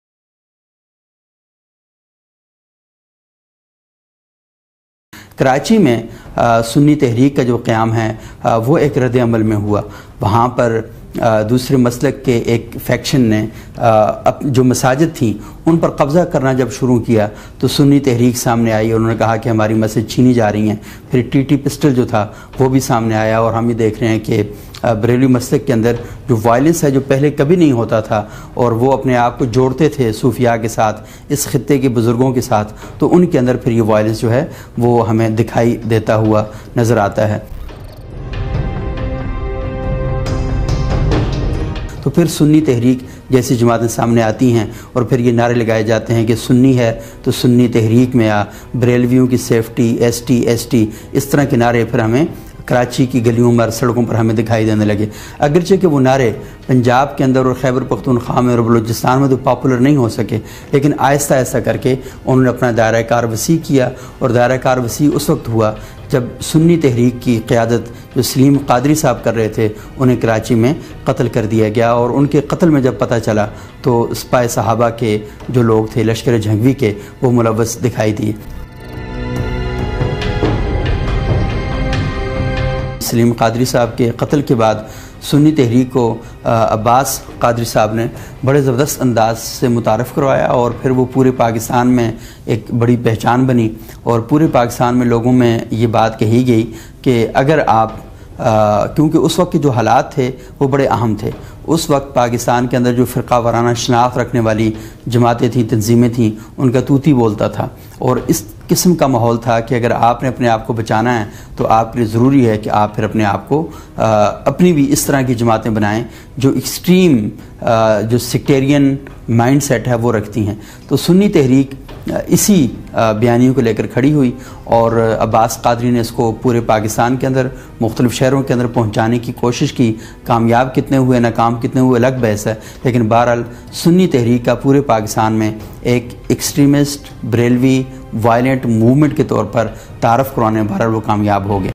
Sunni Tehreek | Barelvi Extremists | Extremist Sunni | Sipah e Sahaba | Sectarianism | Maximist | ST
Karachi Mein Sunni Tehreek Ka Qayam
History of Sunni Tehreek
Sipah e Sahaba Attacks, Bombing and Assassination's of Barelvi Muslims in Nishtar Park Karachi and Other

The assassination of Saleem Qadri gave violent rise to Sunni Tehreek, hanging of Mumtaz Qadri did the same for TLP. Here is an insight into the rise of Barelvi extremism in the recent past.

Sunni Tehreek to TLP: The Rise of Barelvi Extremism | Journalist Sabookh Syed